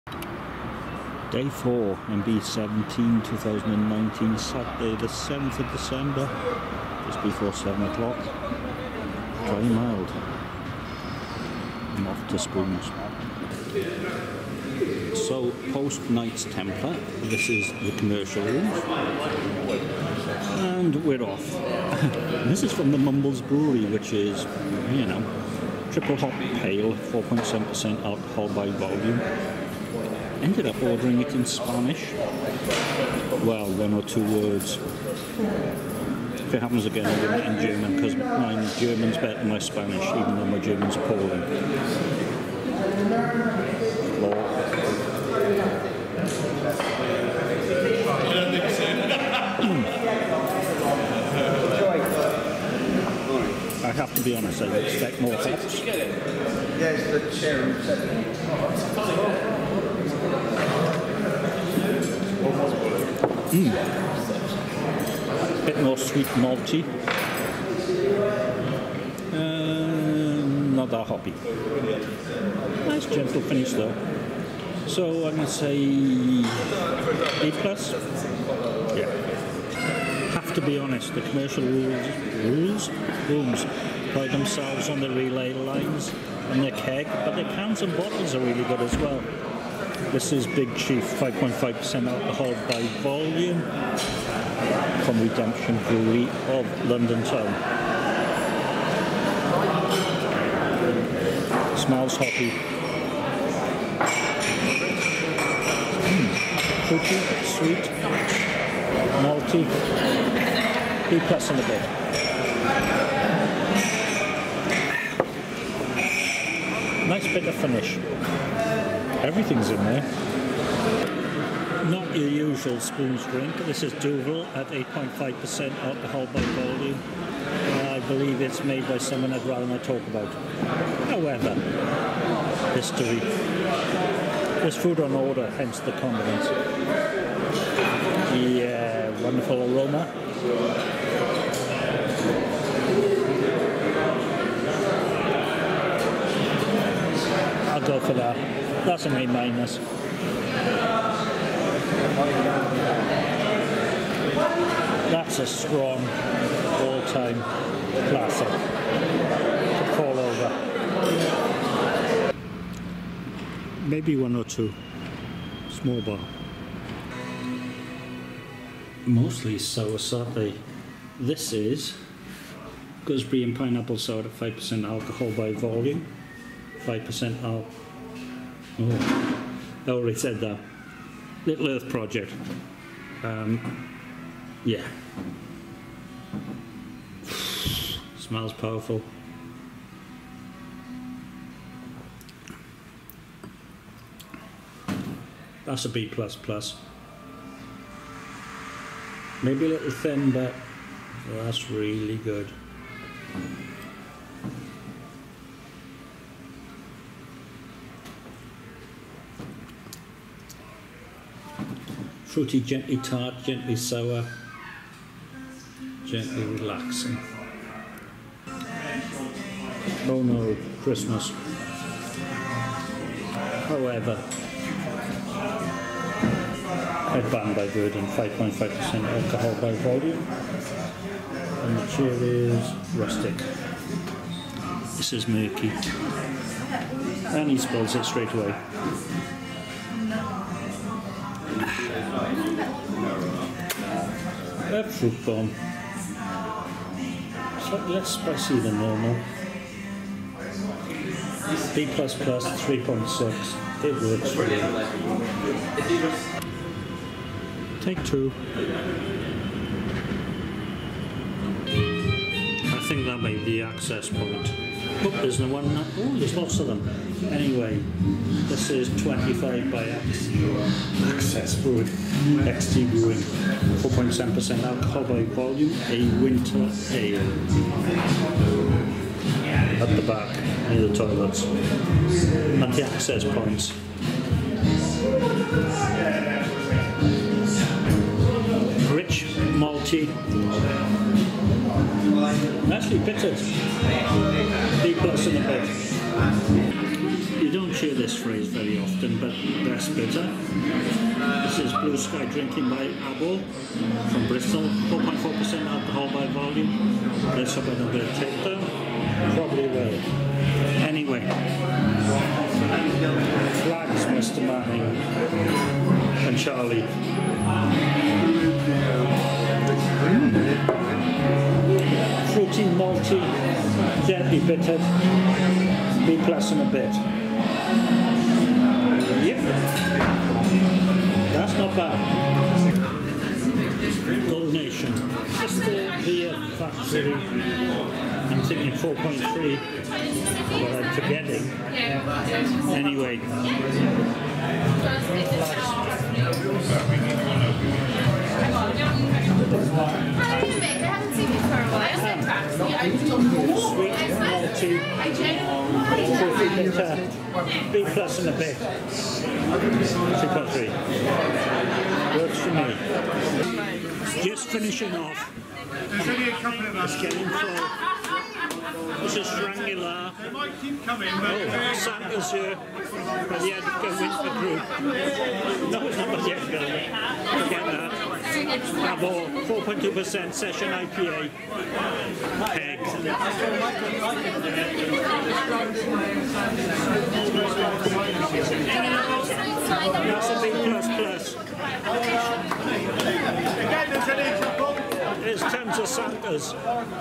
The 2020 Stillage Plan
A mild day for December and you find out that the Commercial Rooms has all the Spoons Festival Ales on Stillage. Then you have too quality plans. Festive Jumpers in the Small Bar and The Bristol Beer Emporium.